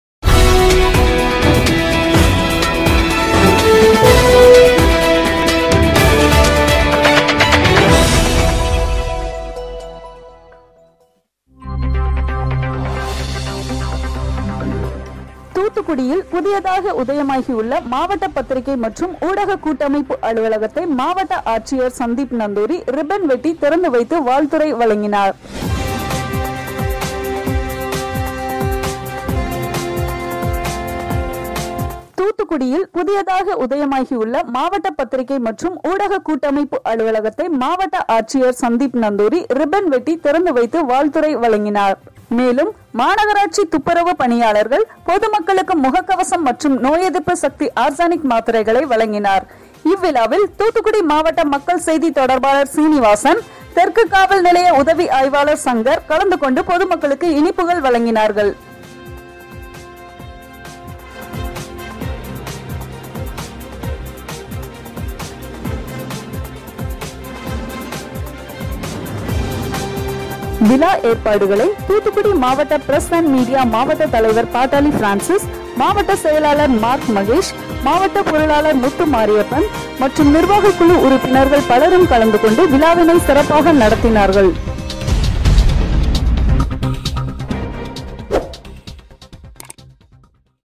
தூத்துக்குடி மாவட்ட பத்திரிக்கை & ஊடக கூட்டமைப்பு அலுவலக திறப்பு விழா/opening Thoothukudi DRMP /
தூத்துக்குடியில் புதியதாக, மாவட்ட பத்திரிக்கை மற்றும் ஊடக கூட்டமைப்பு  அலுவலகத்தை மாவட்ட ஆட்சியர் சந்தீப் நந்தூரி திறந்து வைத்தார்.

தூத்துக்குடியில் புதியதாக உதயமாகியுள்ள மாவட்ட பத்திரிக்கை மற்றும் ஊடக கூட்டமைப்பு  அலுவலகத்தை மாவட்ட  ஆட்சியர் சந்தீப் நந்தூரி ரிப்பன் வெட்டி திறந்து வைத்து, வாழ்த்துரை வழங்கினார்.

மேலும் மாநகராட்சி துப்புரவு பணியாளர்கள், பொதுமக்களுக்கு முகக்கவசம் மற்றும் நோய் எதிர்ப்பு சக்தி ஆர்சனிக் மாத்திரைகளை   வழங்கினார். இவ்விழாவில் தூத்துக்குடி மாவட்ட மக்கள் செய்தி தொடர்பாளர் சீனிவாசன், தெற்கு காவல் நிலைய உதவி ஆய்வாளர் சங்கர் கலந்து கொண்டு பொதுமக்களுக்கு இனிப்புகள் வழங்கினார்கள்.